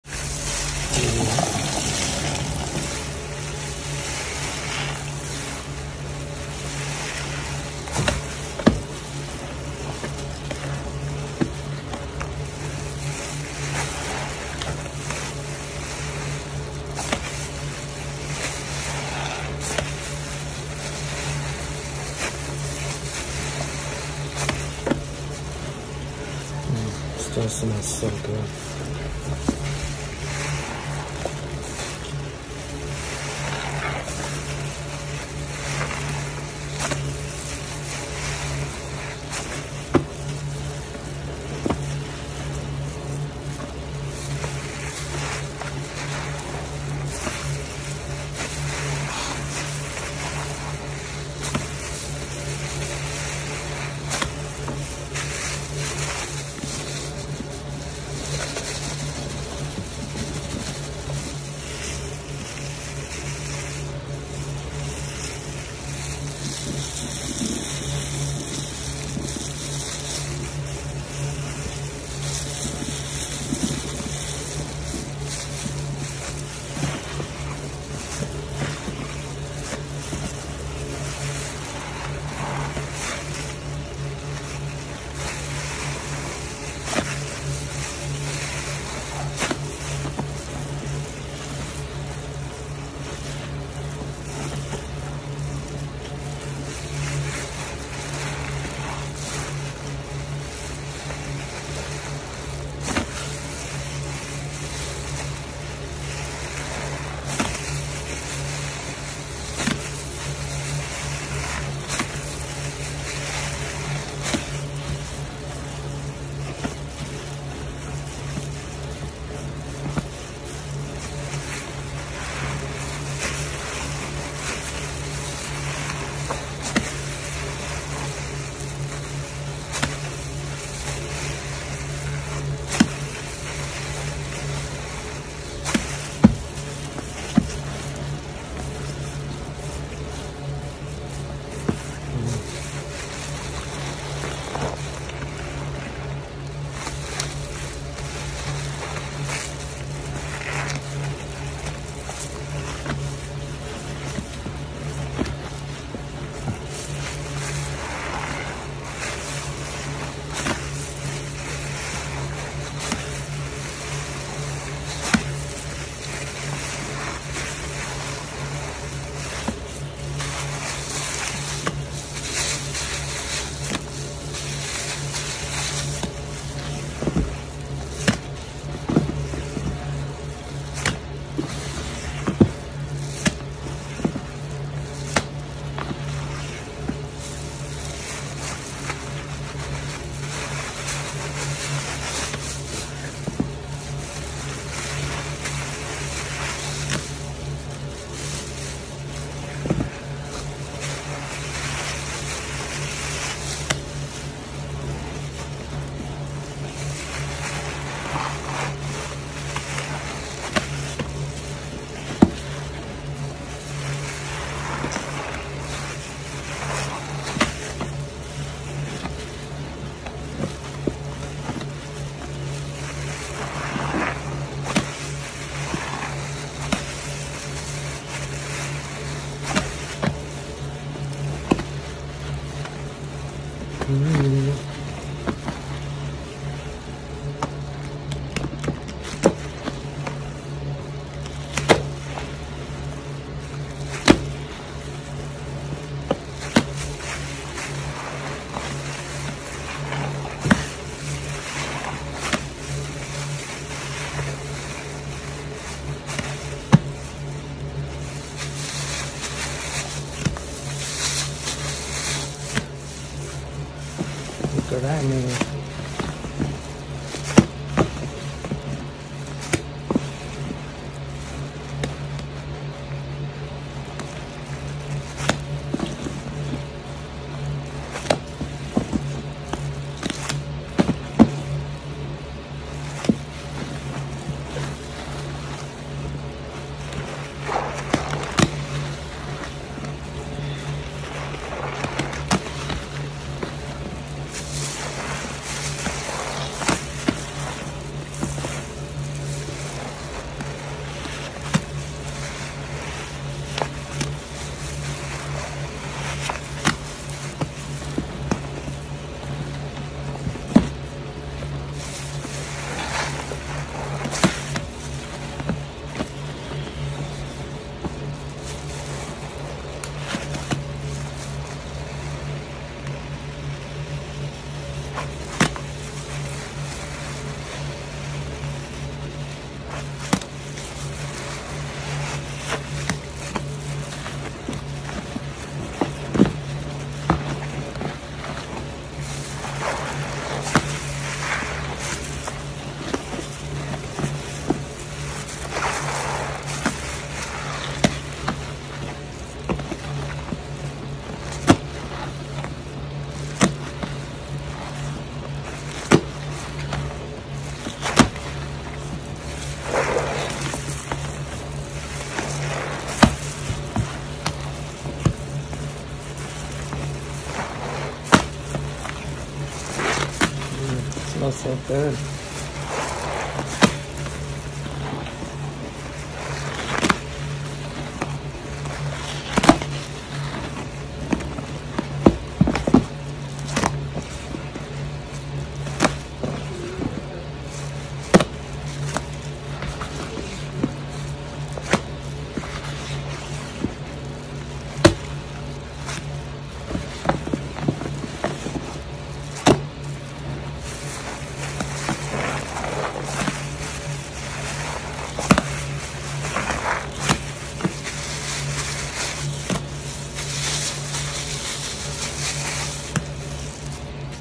My milkshake baby! Thick and juicy
Some soaked soap. Pinalen pink Pinesol just sooooo yummy and thick!